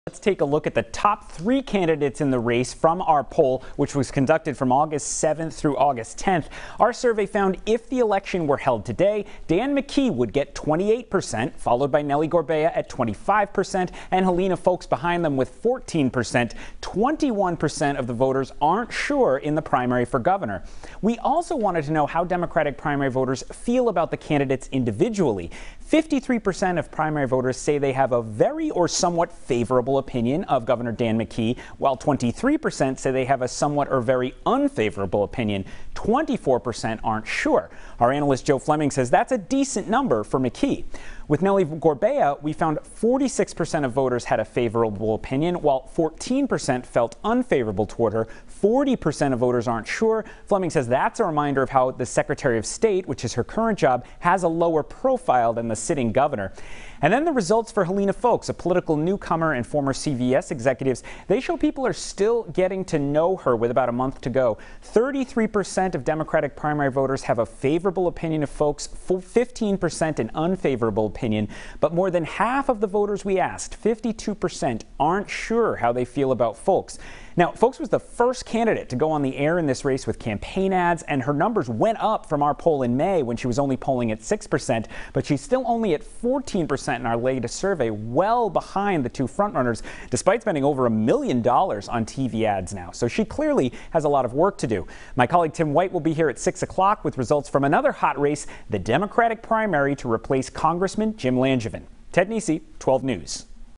12 News/RWU Poll: Primary voters view McKee, Gorbea, Foulkes favorably
Incumbent Gov. Dan McKee is the best known among the top three Democrats running for governor, while Secretary of State Nellie Gorbea and former CVS executive Helena Foulkes are still working to make inroads. Ted Nesi reports.